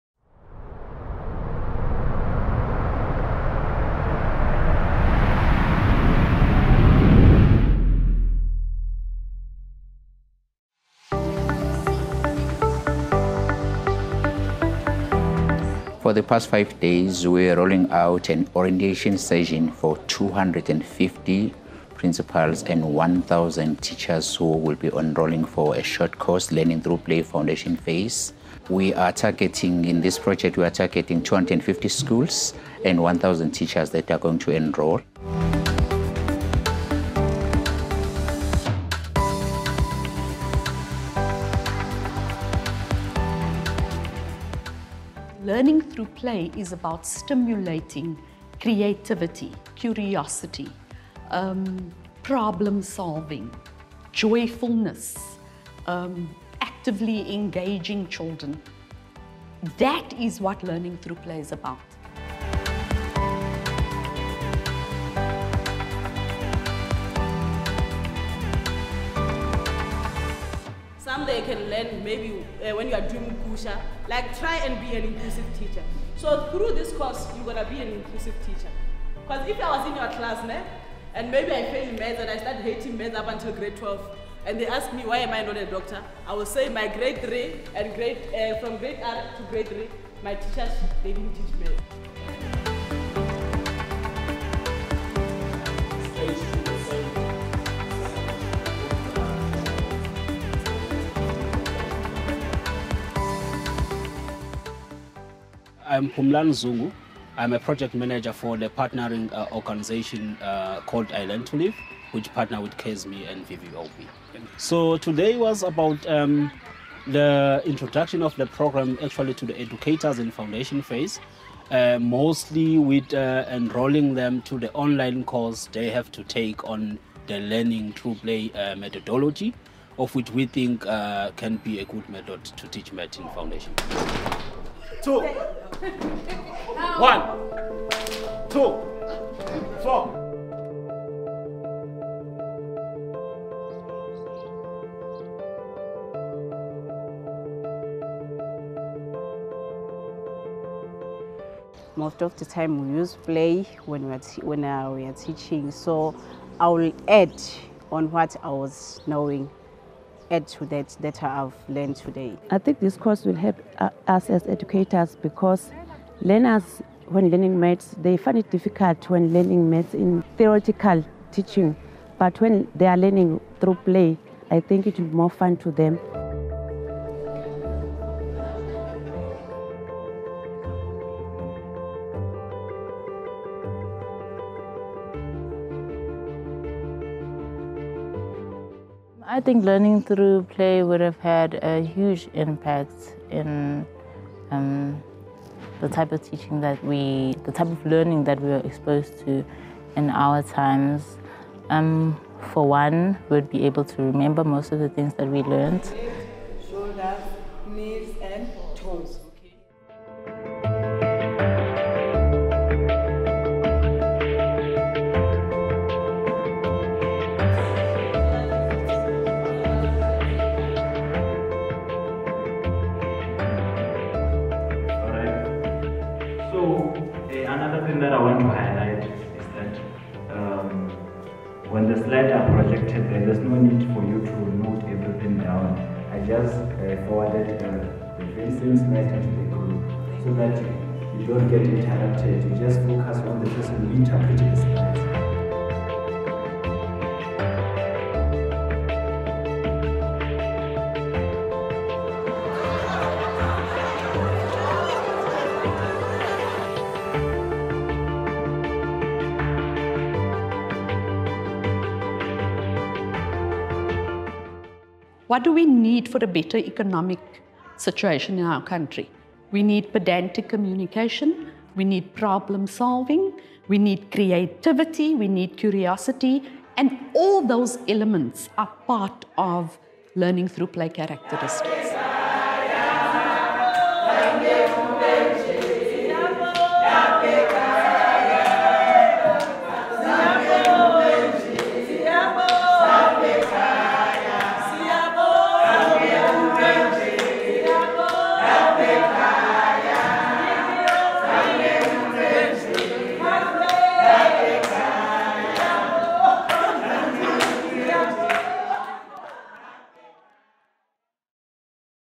VVOB BLEND
VVOB - education for development orientation workshops introducing KwaZulu-Natal educators across 4 districts to the BLEND project. The project is aimed at equipping educators with the neccessary skills and support to incorporate learning through play for learners in the foundation phase.